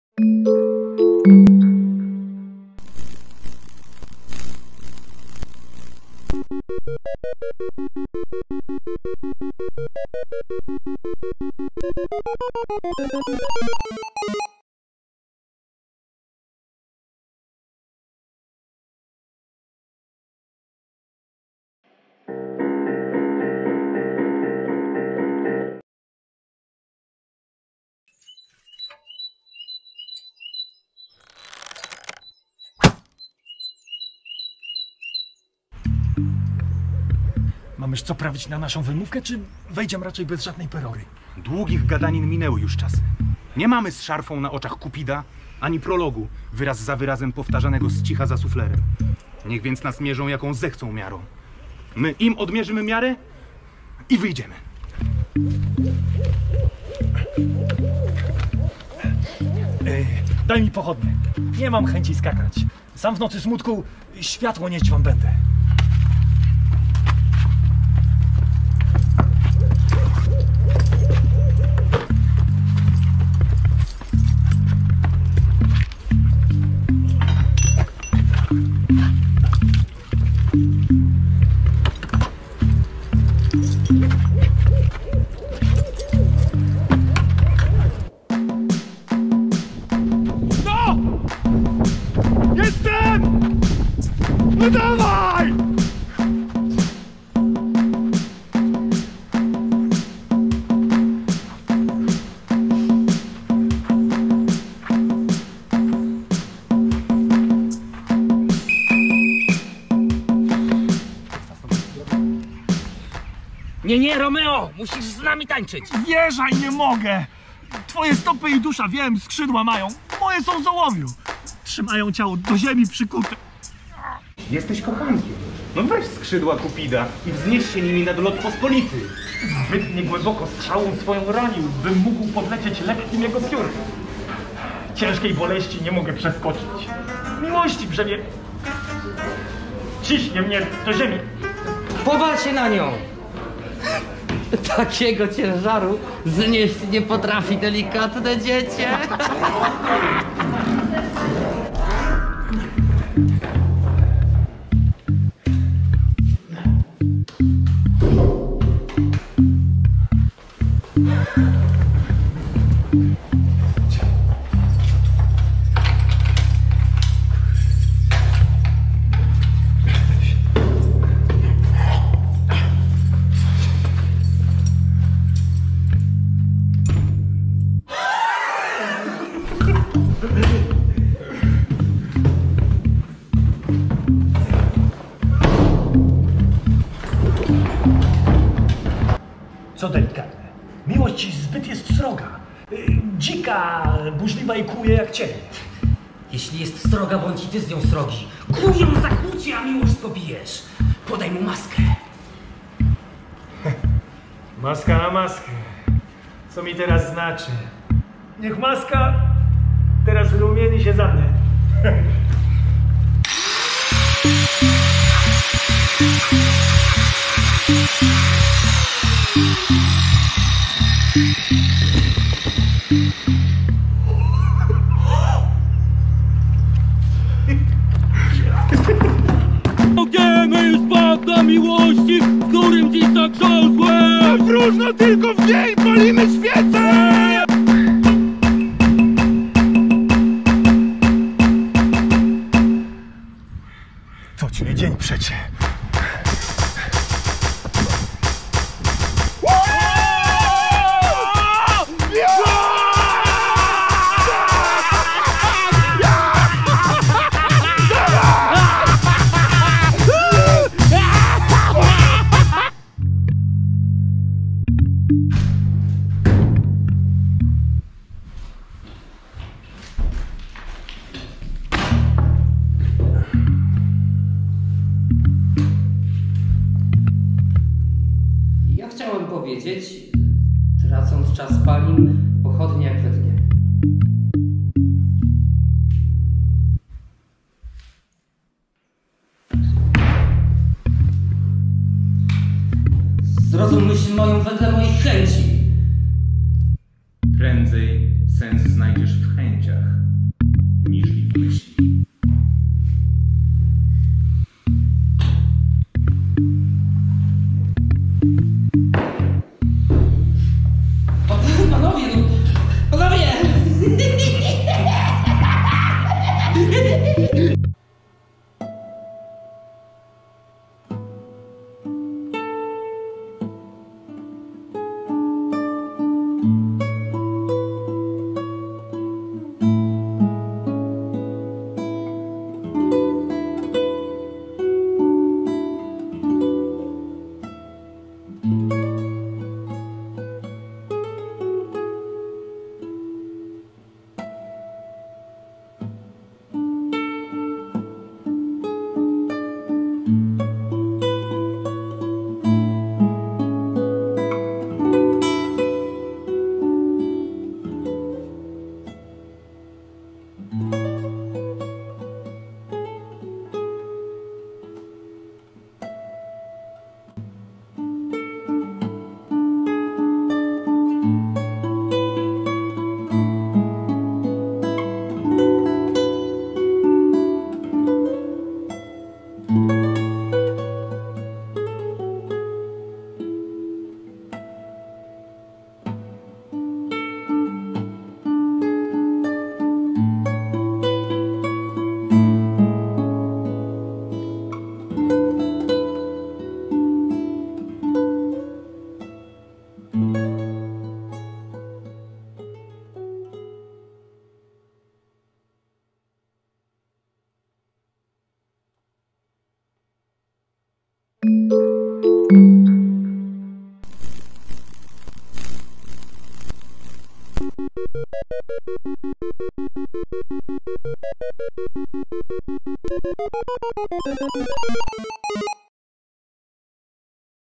ArlekiNowyinternetOWY: Faceci w kryzach - odcinek I - "Na skrzydełkach Kupida"
Prosimy nie regulować odbiorników! Nadchodzą "Faceci w kryzach"!
Dziś pierwszy odcinek czwartej serii cyklu ArlekinNowyinternetOWY, czyli trzech śniących mężczyzn... w mięsnym, w rajtuzach i w objęciach Kupida.
W gęstych oparach absurdu i z dystansem do klasyki. :)

Teatr Lalek Arlekin oraz Wojtek Kondzielnik, Maciej Piotrowski i Wojciech Schabowski